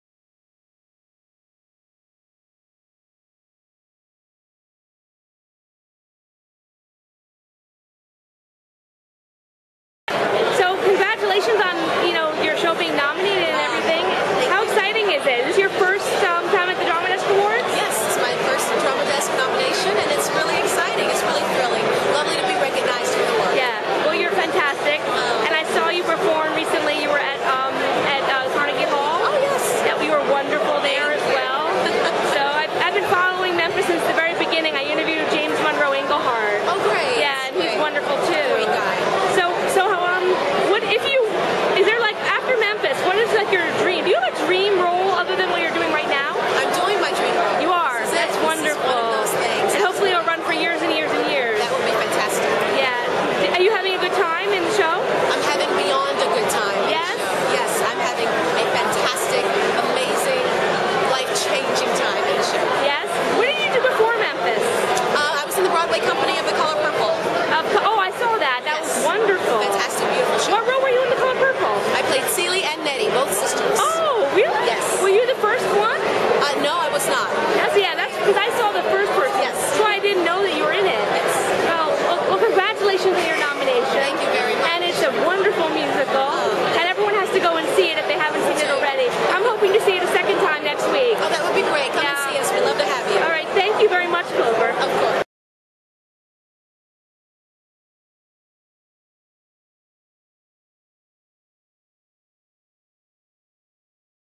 Interview with Montego Glover from MEMPHIS ON BROADWAY
MONTEGO GLOVER AT THE DRAMA DESK NOMINATIONS PARTY chats with Corine Cohen.